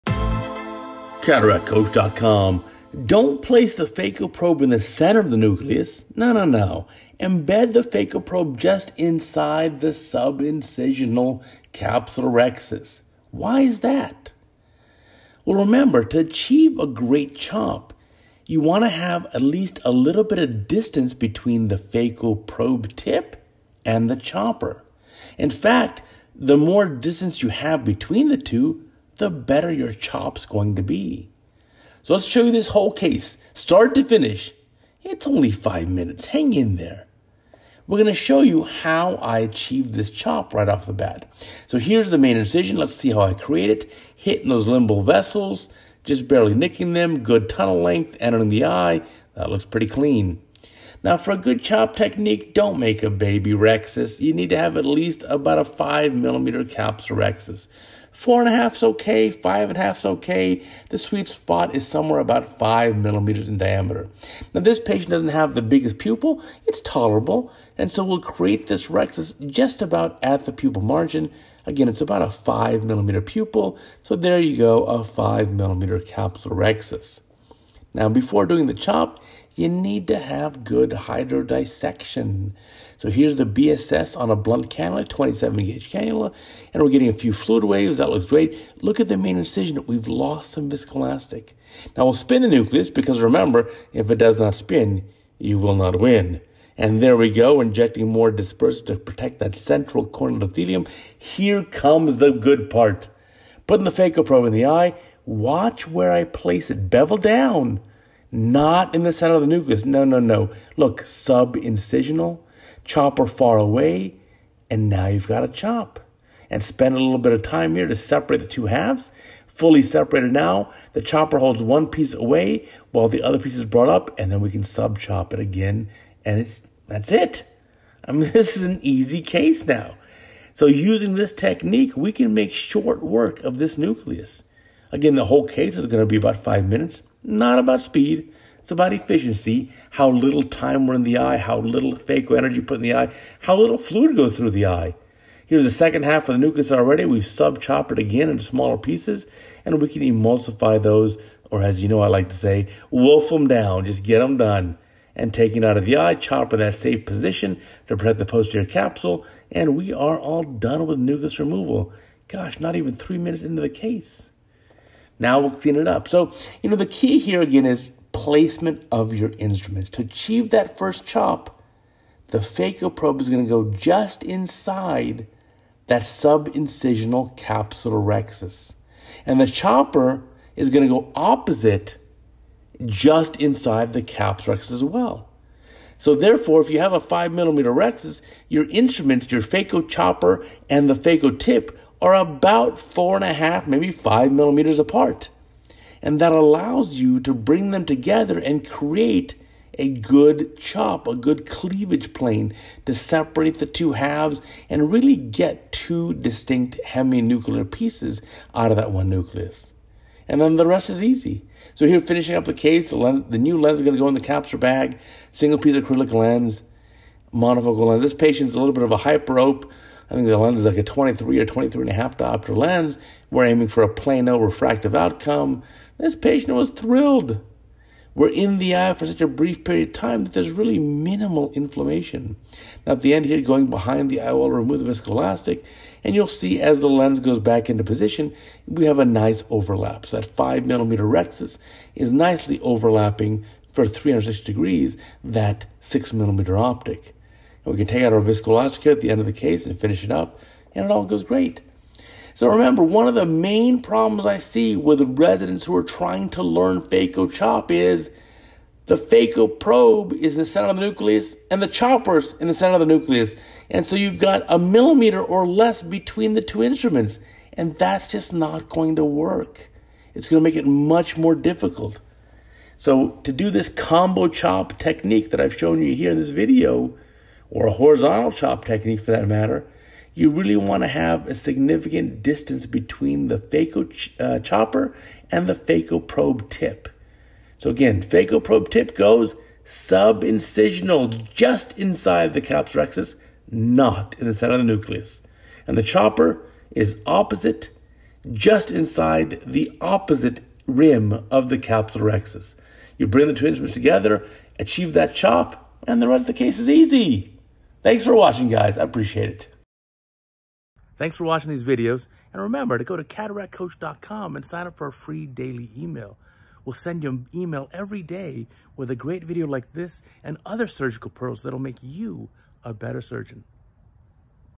CataractCoach 1187: Don't place probe in center for combo chop
To achieve the best combo chop, you want to have a sufficient distance between the phaco probe and the chopper tip so that you can create a complete separation of the nucleus. This means that you do not want to place your probe in the center of the nucleus, but rather just inside the sub-incisional capsulorhexis. This will allow you to make short work of nucleus removal and complete your case in an efficient manner.